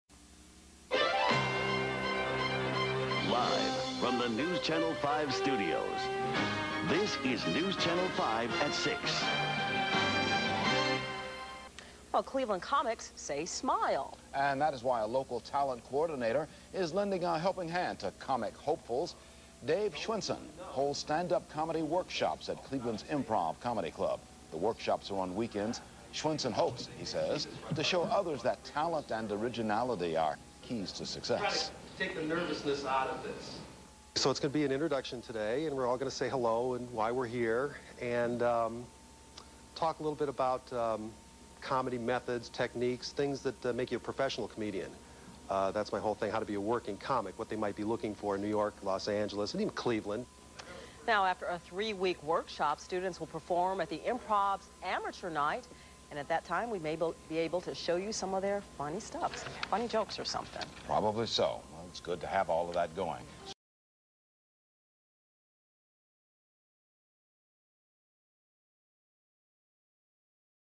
Comedy Workshop at The Cleveland Improv
Dave Schwensen's comedy workshops at The Cleveland Improv. Dave is the author of "How To Be A Working Comic" and "Comedy FAQs